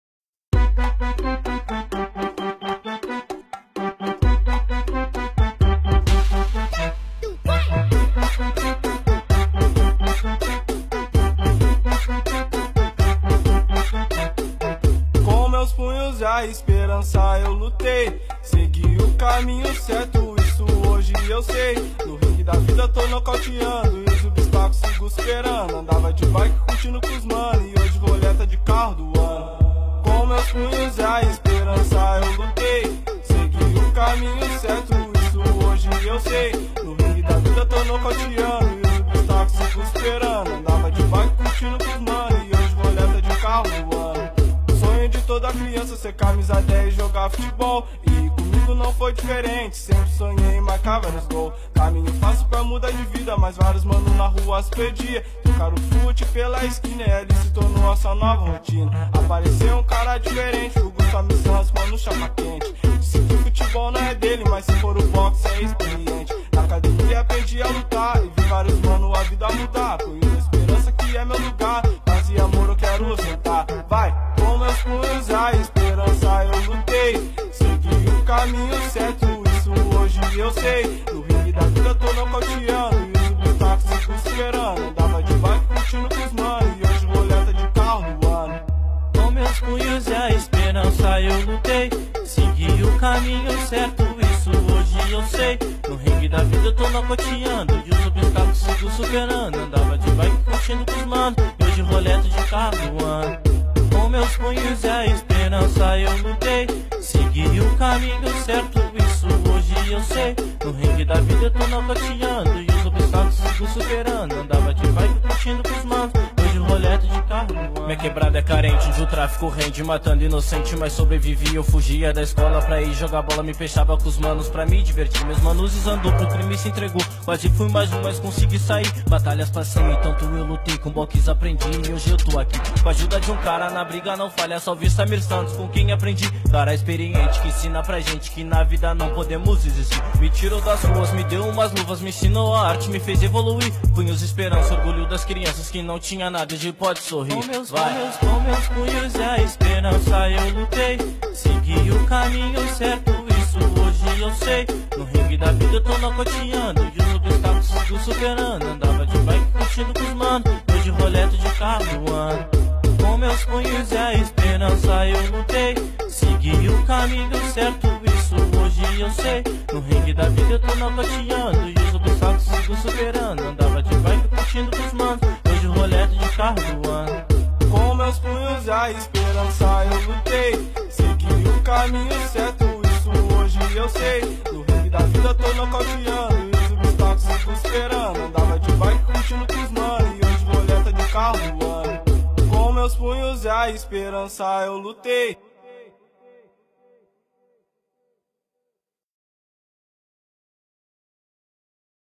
Música em homenagem a Samir Santos
HOMENAGEM DE AGRADECIMENTO A MIM E AO PROJETO É SEMPRE ALGO QUE ME DEIXA MUITO FELIZ..Agradecimemtos aos meus parças e grandes músicos talentos da nossa cidade Pablo Garcia e Petryck Jhordan